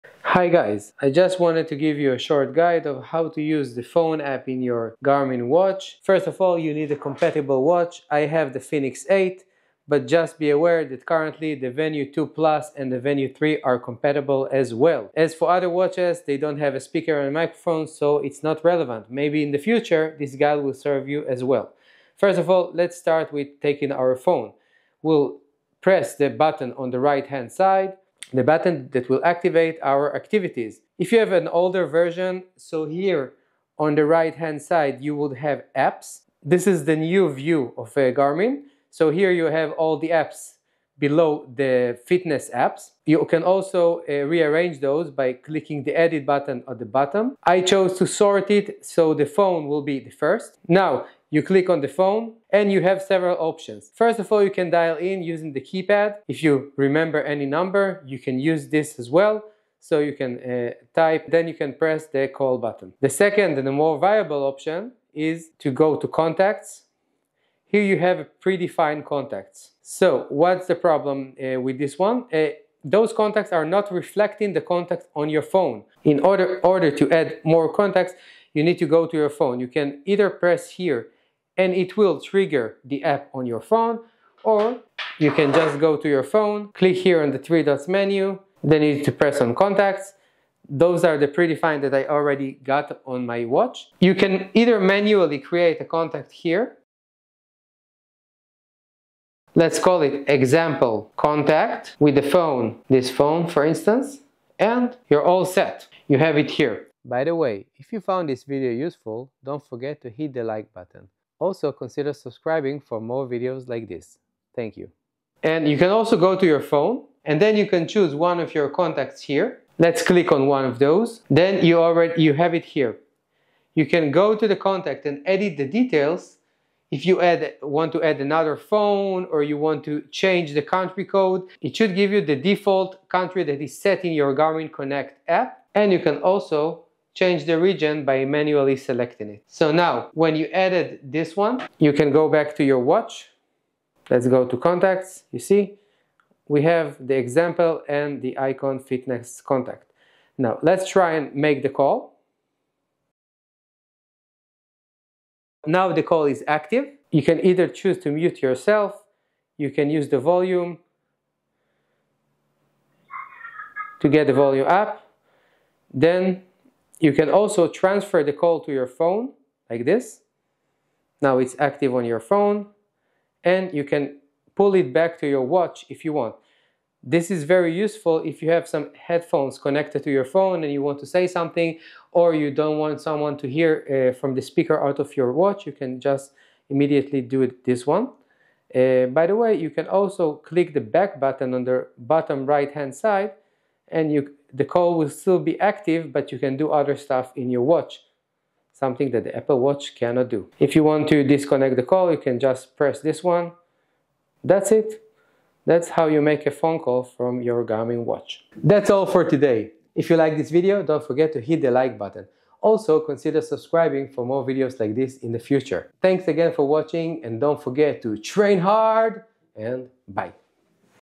How to use the Garmin Phone App and make calls, contacts and more
How to Use the Garmin Phone App – Make Calls, Access Contacts & More

Did you know your Garmin smartwatch can do more than track workouts? In this video, I’ll show you how to use the Garmin phone features, including how to make calls, access contacts, and manage smart notifications right from your wrist.

✅ In this video, you’ll learn:
How to set up the Garmin Connect and Garmin Smartphone Link apps
How to sync contacts to your Garmin watch
How to make and receive phone calls (for supported models)
How to view and respond to messages and notifications
Which Garmin watches support phone features

📱 Compatible with:
Garmin Venu, Forerunner, Fenix, Epix, and others (with phone call support)
Android & iOS devices